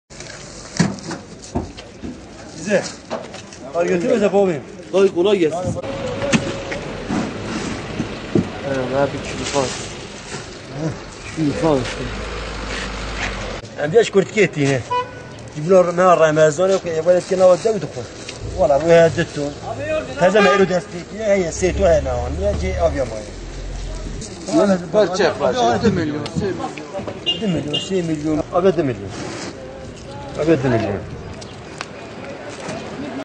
Muşlular iftarlarını kar ile açacak! - 09 Temmuz 2013
Muşlular, Ramazan ayının ilk orucunu kar ile açacak.

Değerli İzleyicilerimiz;

TÜRKÇE

KÜRTÇE-KURMANCI

ARAPÇA

İNGİLİZCE

FARSÇA

KÜRTÇE-SORANİ